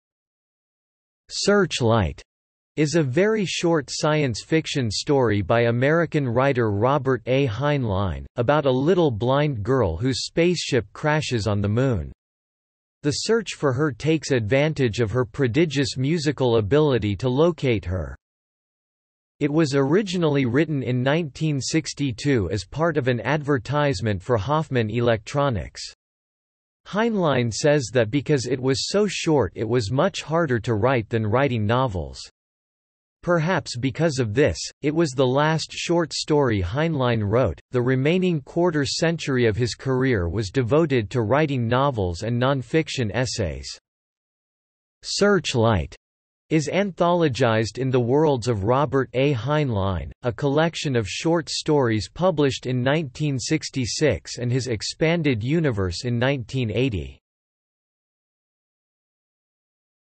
Searchlight (short story) | Wikipedia audio article
- Socrates

SUMMARY
"Searchlight" is a very short science fiction story by American writer Robert A. Heinlein, about a little blind girl whose spaceship crashes on the Moon. The search for her takes advantage of her prodigious musical ability to locate her.
It was originally written in 1962 as part of an advertisement for Hoffman Electronics.  Heinlein says that because it was so short it was much harder to write than writing novels.  Perhaps because of this, it was the last short story Heinlein wrote; the remaining quarter-century of his career was devoted to writing novels and non-fiction essays.
"Searchlight" is anthologized in The Worlds of Robert A. Heinlein, a collection of short stories published in 1966 and his Expanded Universe in 1980.